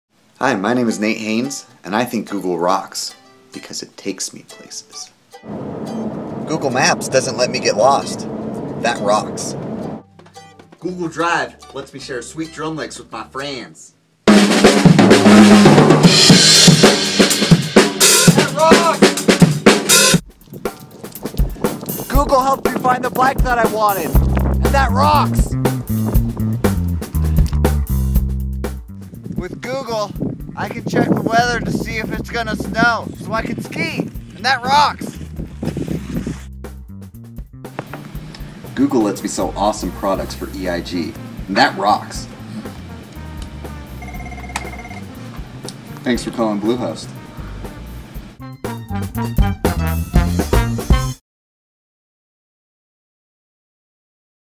Google Rocks
This is my entry for the EIG "Google Rocks" competition. I decided take video of all the places/activities that I use Google for. Enjoy!